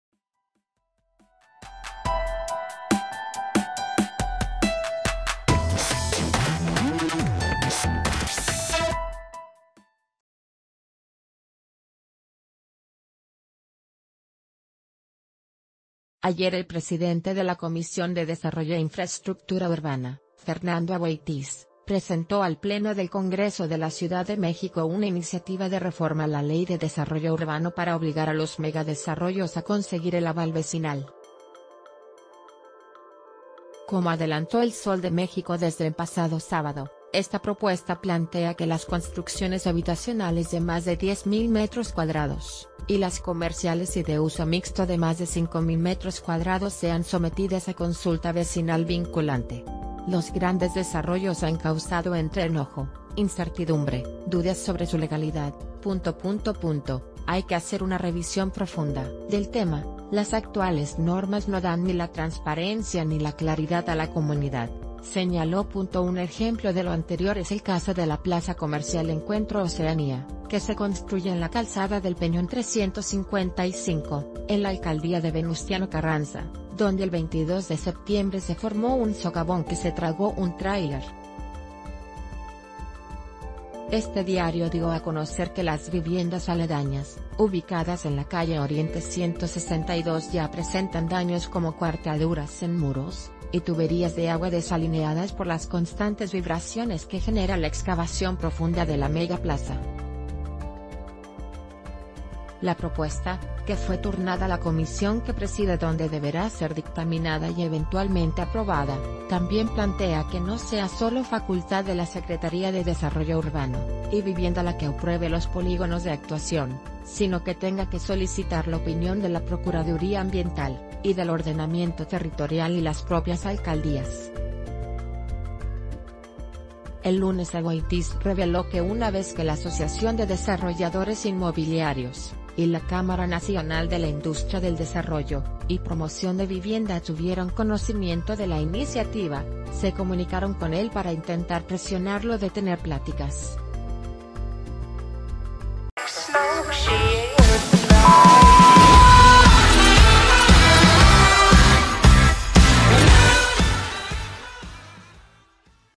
Buscan freno a daños colaterales, vecinos deberán avalar megaobras en CDMX
Ayer el presidente de la Comisión de Desarrollo e Infraestructura Urbana, Fernando Aboitiz, presentó al Pleno del Congreso de la Ciudad de México una iniciativa de reforma la Ley de Desarrollo Urbano para obligar a los megadesarrollos a conseguir el aval vecinal. Como adelantó El Sol de México desde...